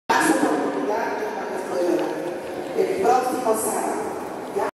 video al pie de Sta Teresita dsps misa de canonizacion de sus padres SAM 2625
MISA SOLEMNE DE CANONIZACION DE LOS PADRES DE STA TERESITA DEL NIÑO JESUS Y DE LA SANTA FAZ
Jacqueline Villacorta (voz y Guitarra) momentos al finalizar la celebración litúrgica.